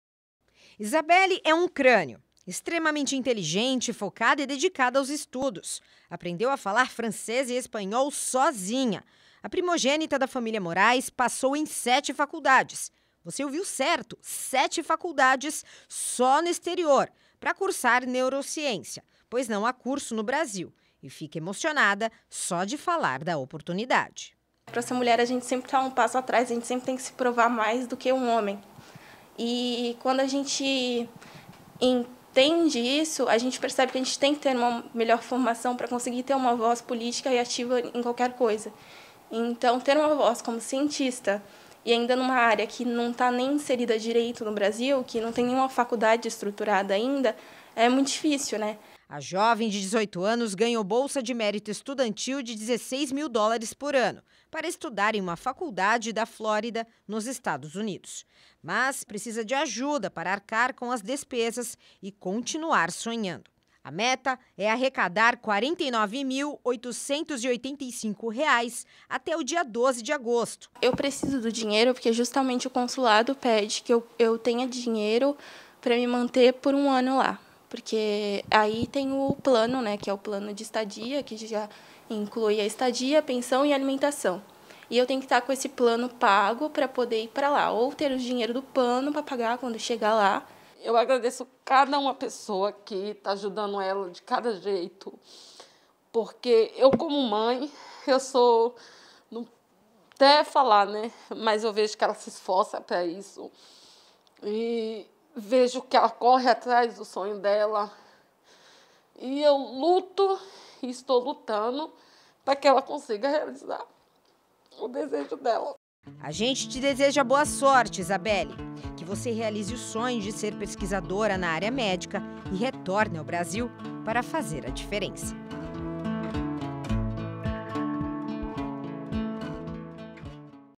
Jovem de Cubatão ganha bolsa de estudos fora do país e cria vaquinha para ajudar nas despesas
Uma jovem de Cubatão ganhou uma bolsa para estudar fora do país, mas para ajudar a arcar com as despesas, ela criou uma vaquinha virtual.